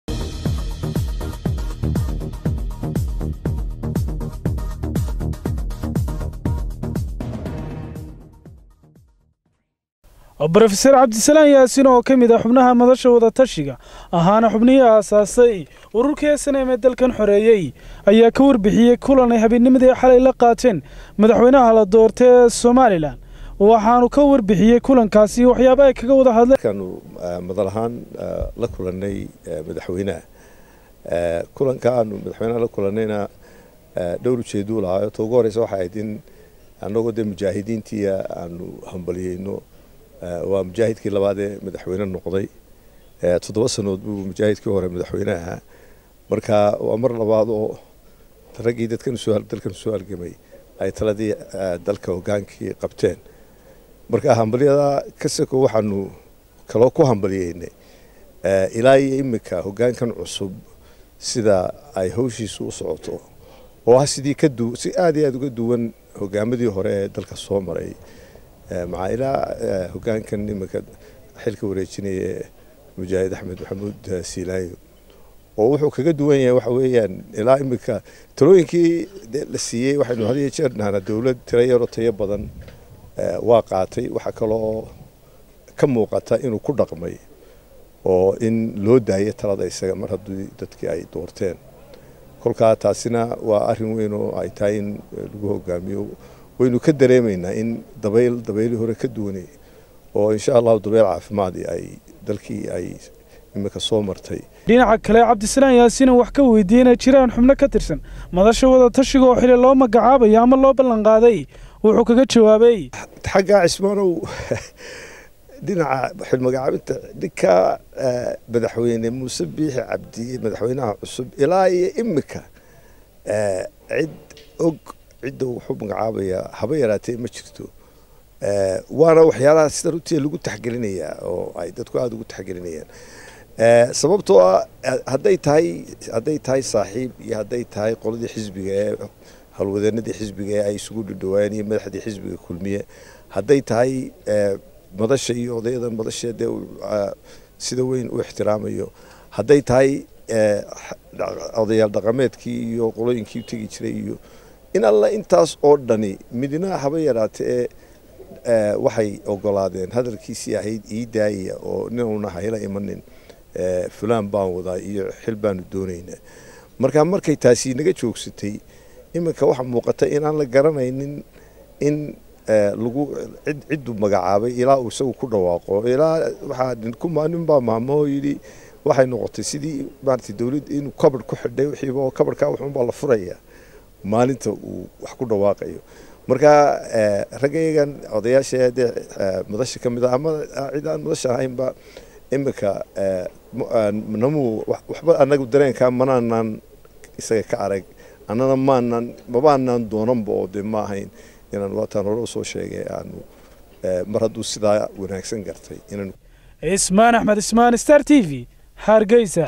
DAAWO MADASHA WADATASHIGA OO KA HADASHAY KULAN AY MUUSE BIIXI LA QATEEN +IN XILAL LO BALANQAADAY
★ ★ ★  Ku Xidhnow: Somali✔  startv★ ★ ★